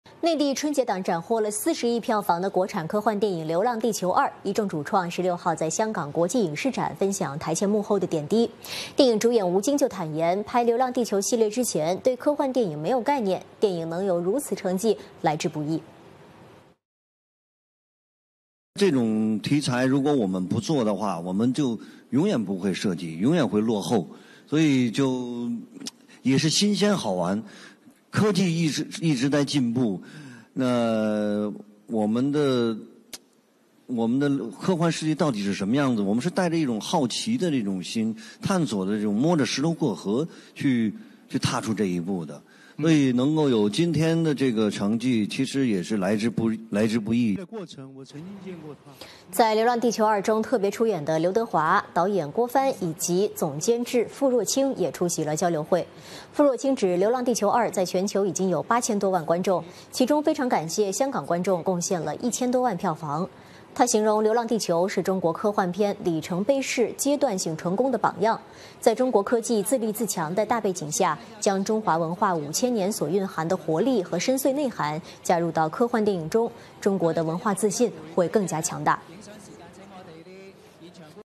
【一眾主創香港分享幕後點滴】吳京談《流浪地球2》：成績來之不易
內地春節檔斬獲40億票房的、國產科幻電影《流浪地球2》一眾主創、16日在香港國際影視展分享台前幕後點滴。電影主演吳京坦言，拍《流浪地球》系列前對科幻電影沒有概念，電影能有如此成績來之不易。